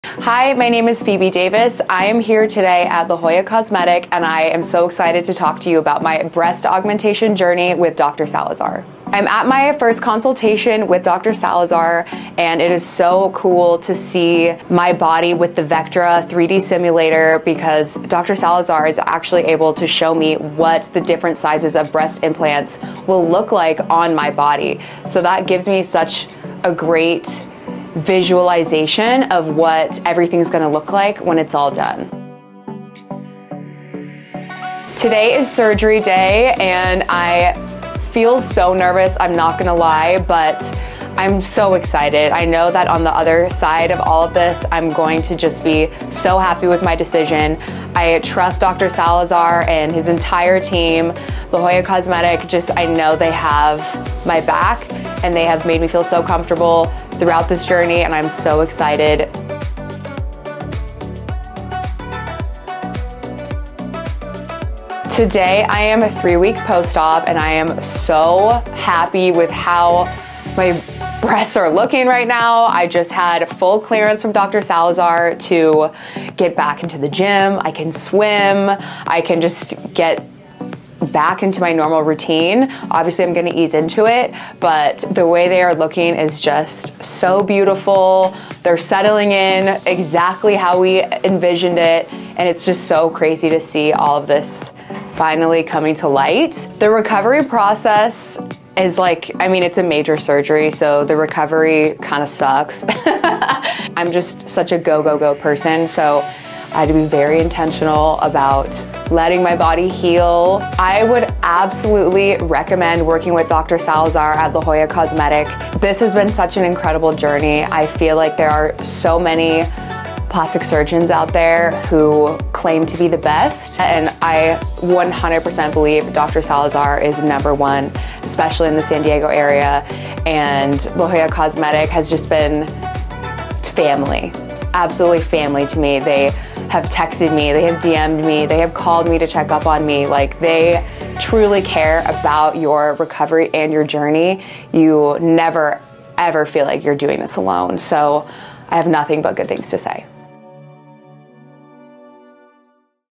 Phoebe Davis’ Breast Augmentation Experience at La Jolla Cosmetic
Phoebe Davis, model, influencer, and star of Amazon Prime’s reality dating series Cosmic Love, chose La Jolla Cosmetic’s renowned breast surgeon Dr. Hector Salazar-Reyes to help her achieve her dream breasts. We documented every part of the process so you can get a behind-the-scenes look at what it’s like to get a breast augmentation here at our cosmetic surgery centre!

She opens up with us about her feelings at every step of her experience, from the decision-making process, to her procedure day, to the recovery process, and finally to her excitement over her final results!

Dr. Salazar is the #1 breast surgeon in the San Diego area in Phoebe’s books, from his personality and style of patient care to the fabulous results he gave her. She truly felt like he and the rest of our team made her feel like family. We are so glad she trusted us to make her vision a reality.

View before and after photos of breast augmentations Dr. Salazar has done

Stream Cosmic Love on Prime Video

Contents of this video:

00:00 - Welcome Phoebe!

00:10 - My first breast augmentation consultation

00:16 - Vectra 3D simulator for the breast augmentation consultation

00:34 - My breast augmentation surgery day

01:04 - How I'm feeling 3 weeks after my breast augmentation

01:31 - How my breast implants are settling in

01:40 - How is the recovery after breast augmentation?

01:50 - Why I recommend choosing Dr. Salazar at La Jolla Cosmetic